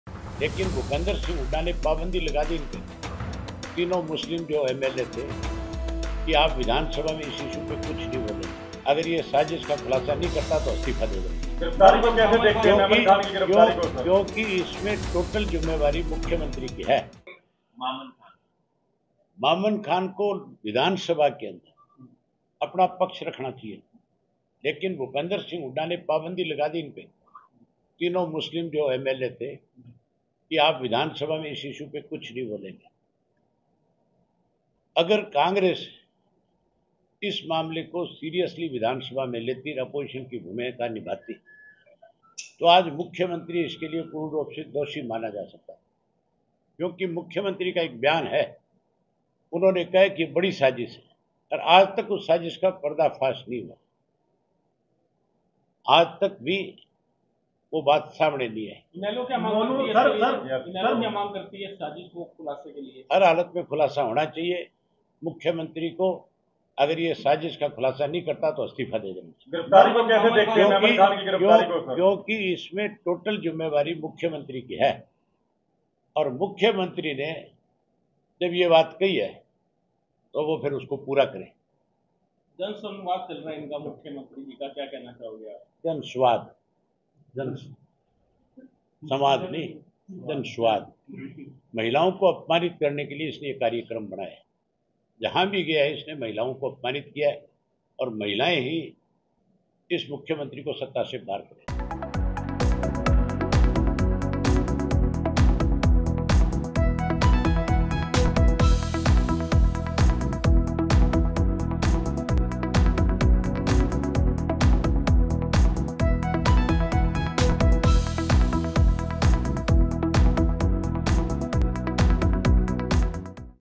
Mamman Khan की गिरफ्तारी पर बोले Abhay,'CM दोषी मिलते अगर Hooda मेवाती विधायकों का साथ देते'।
Mamman Khan की गिरफ्तारी नहीं होती,अगर Bhupinder Hooda मेवाती विधायकों का साथ देते तो,इस मामले में CM दोषी मिलते- Abhay Chautala ने कहा.